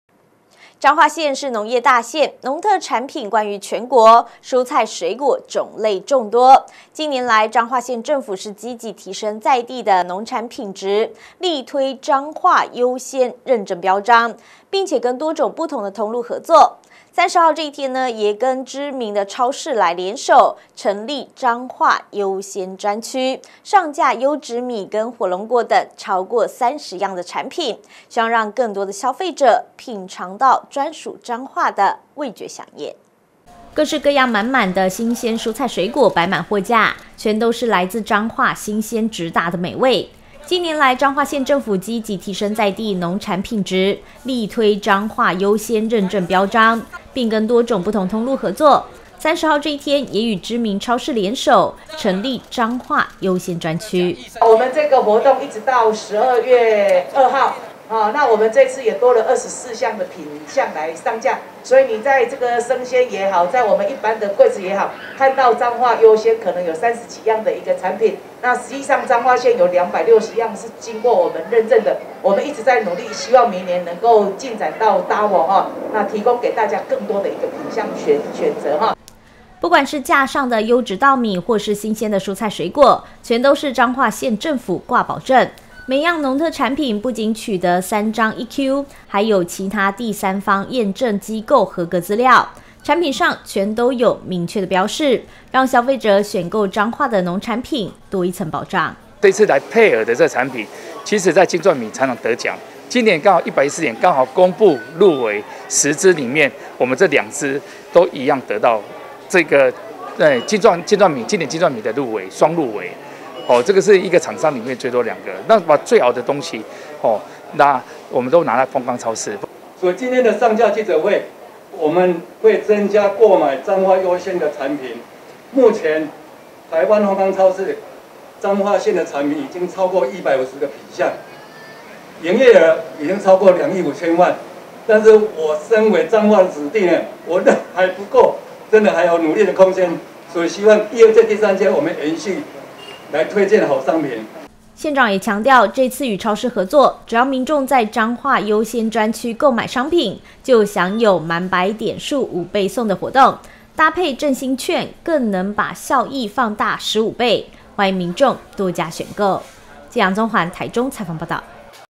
彰化推優鮮認證！超市專區輕鬆買得到-大台中新聞
彰化縣是農業大縣，農特產品冠於全國，蔬菜、水果種類眾多，近年來彰化縣政府積極提升在地農產品質，力推「彰化優鮮」認證標章，並與多種不同通路合作，30號這天也與知名超市聯手，成立彰化優鮮專區，上架優質米與火龍果等超過30樣的產品，希望讓更多消費者品嘗到專屬彰化的味覺饗宴   。「以上新聞可由top NEWS APP收看」